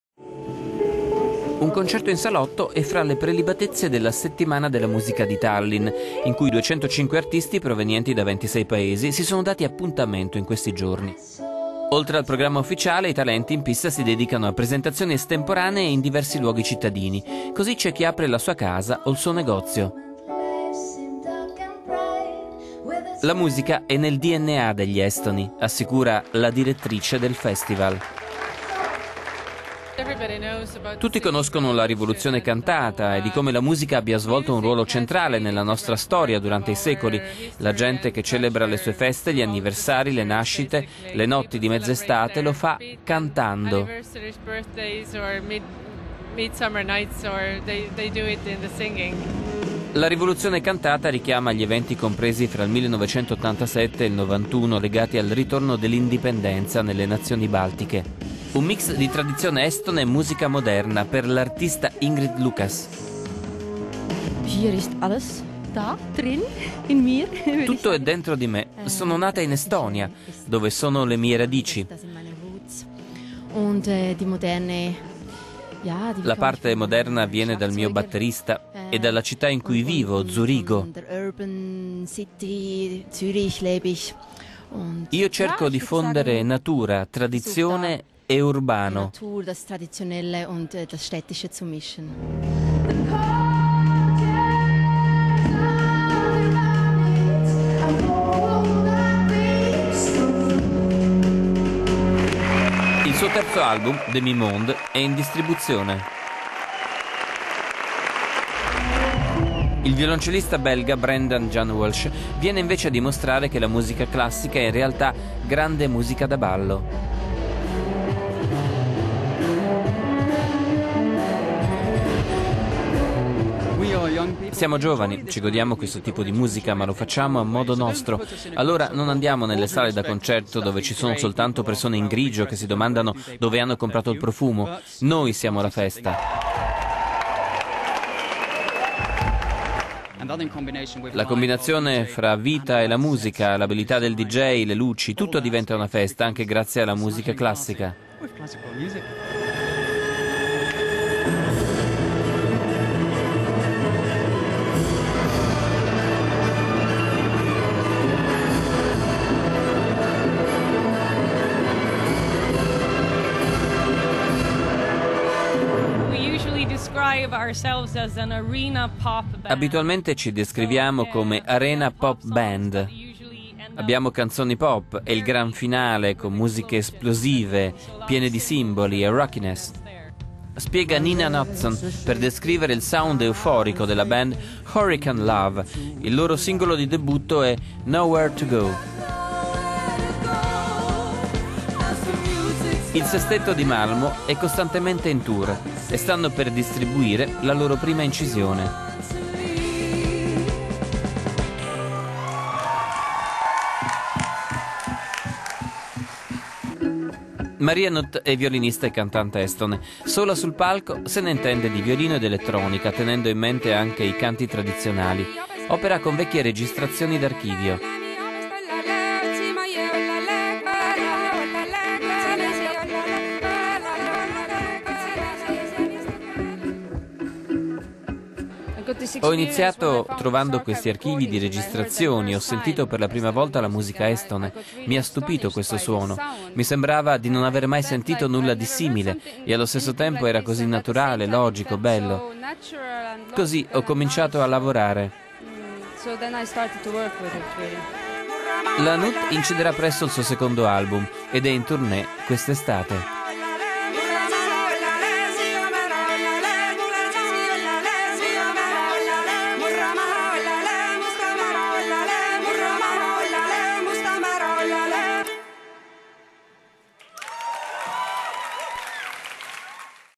Suoni d'Estonia: alla Settimanma della Musica di Tallin - le mag
Un concerto in salotto è fra le prelibatezze della settima edizione della Settimana della Musica di Tallin (Estonia) in cui 205 artisti provenienti da 26 paesi si sono dati appuntamento in questi giorni.

Oltre al programma ufficiale i talenti in pista si dedicano a presentazioni estemporanee in diversi luoghi della città. Cosi' c'è chi apre la sua casa o il suo negozio per ospitare un evento e il suo pubblico limitato  solo dallo spazio disponibile.